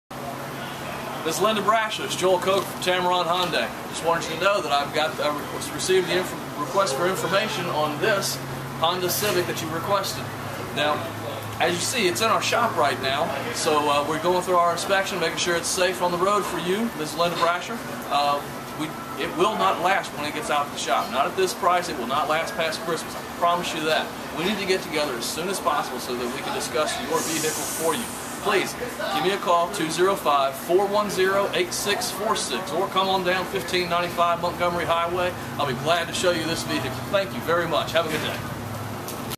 A personal message from Joel Coker at Tameron Hyundai
At this time, this car is available for you. At your convenience, give me a call. I look forward to speaking to you soon. Have a good day.

Joel Coker
Tameron Hyundai